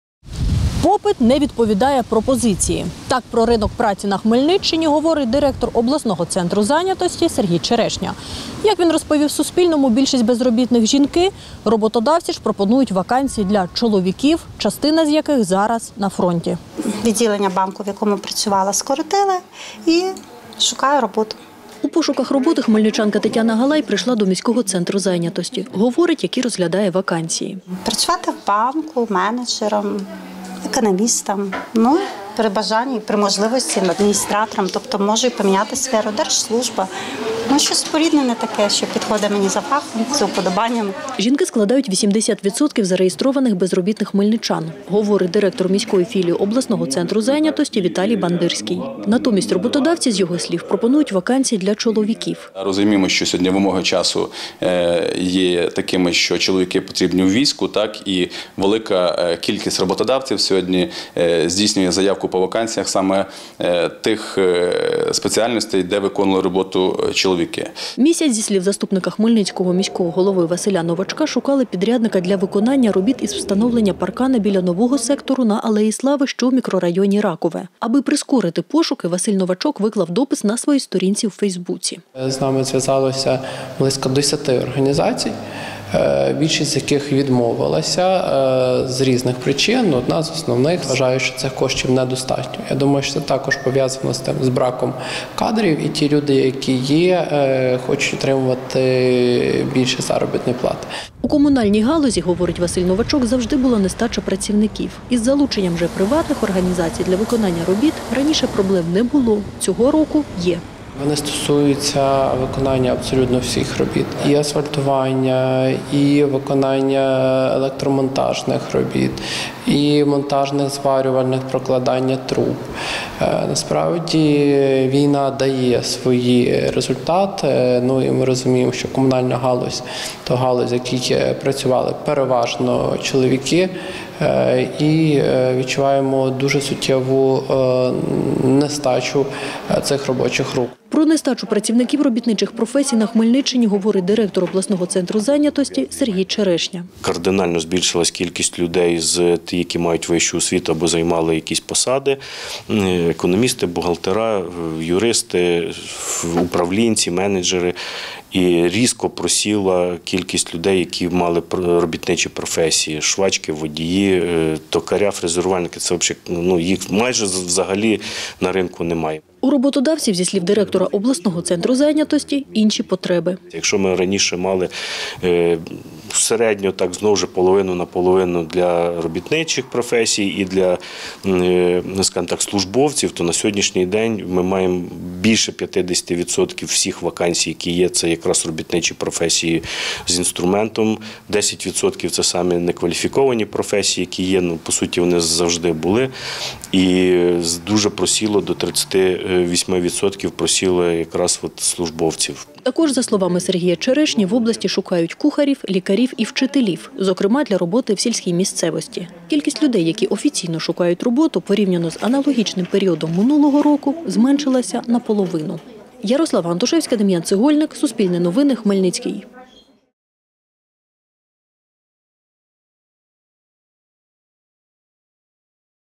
На Хмельниччині попит на ринку праці не відповідає пропозиції
Попит не відповідає пропозиції. Так про ринок праці на Хмельниччині говорить директор обласного центру зайнятості Сергій Черешня. Як він розповів Суспільному, більшість безробітних – жінки, роботодавці ж пропонують вакансії для чоловіків, частина з яких зараз на фронті. Подробиці – в нашому наступному матеріалі.